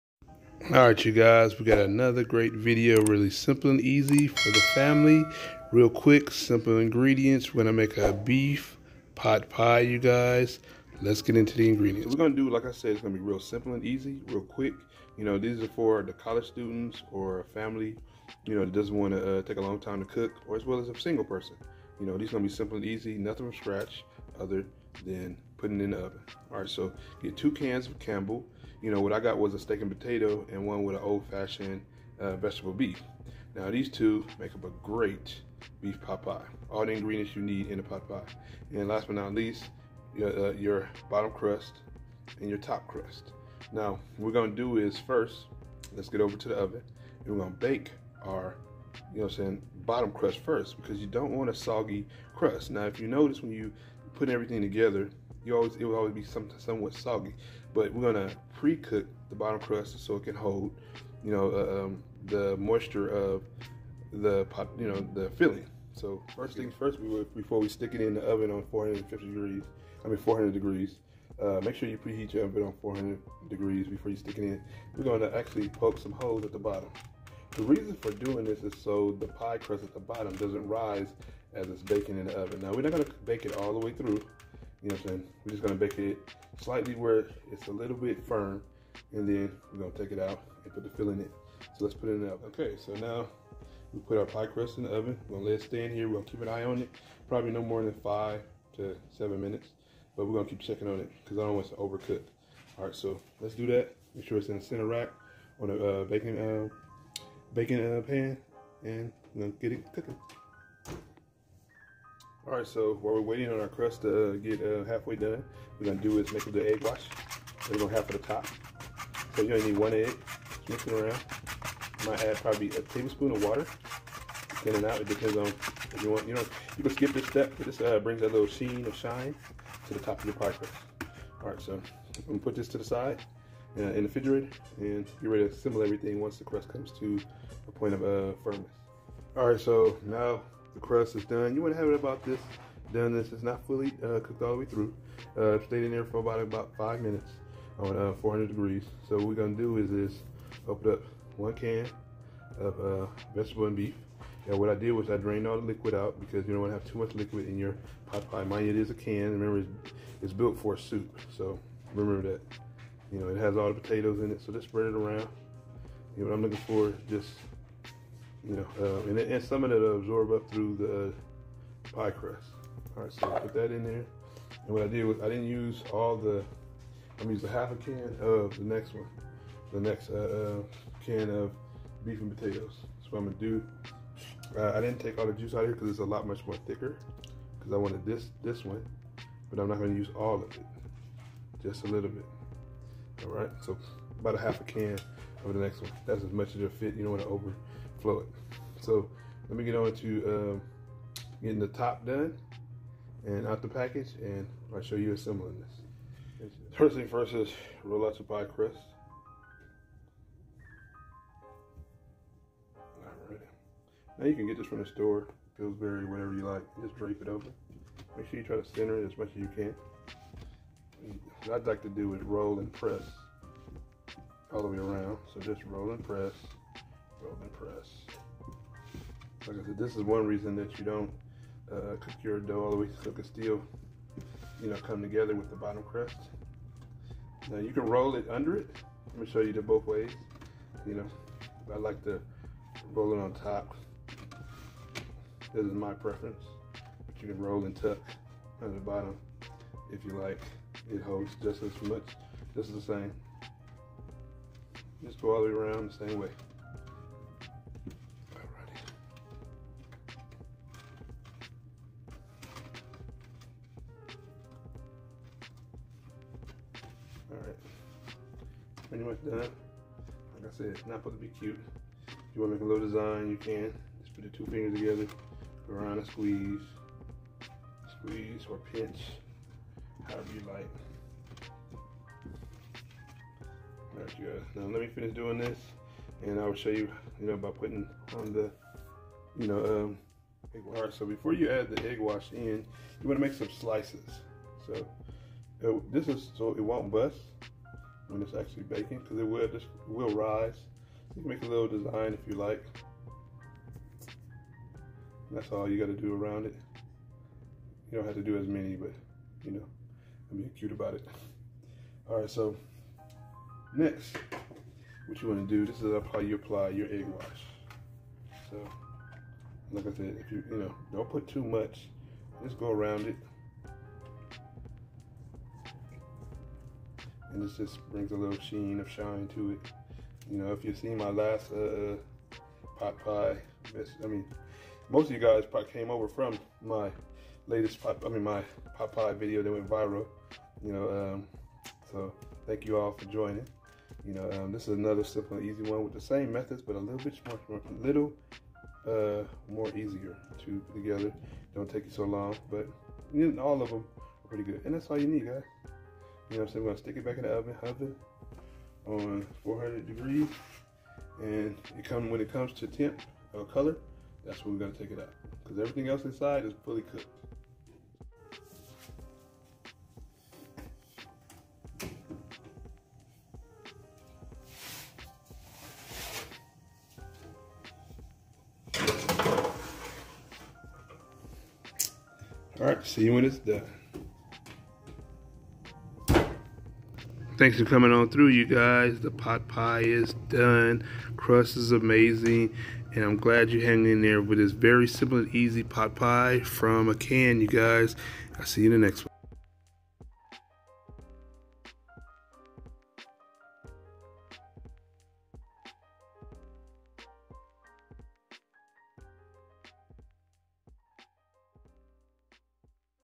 Pot Pie | Easiest 3 Step Beef pot pie Recipe
3 steps to make an easy beef pot pie.

ingredients shopping list

2 cans Campbell beef and vegetables
1 Pillsbury Pie pan
1 Pillsbury Pie crust for Top
1 egg

KC's Amazon Online Store (My Favorite Cooking Essential)